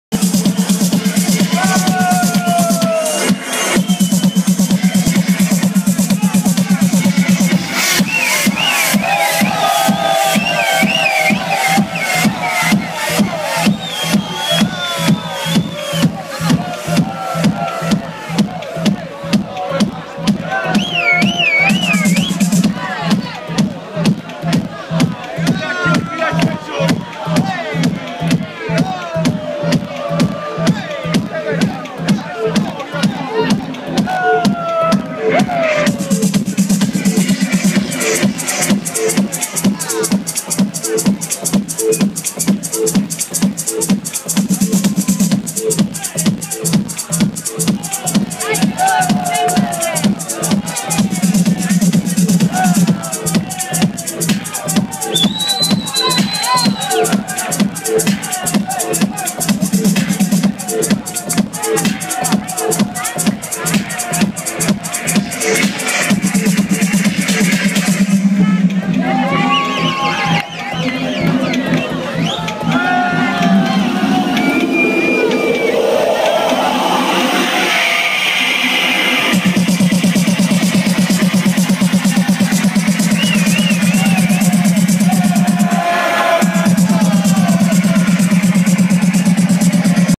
Gigi Squillante Sabato 29 giugno 2013 2*
Gigi Squillante live-set So Much @ Dolce Vita Salerno Sabato 29 giugno 2013 2*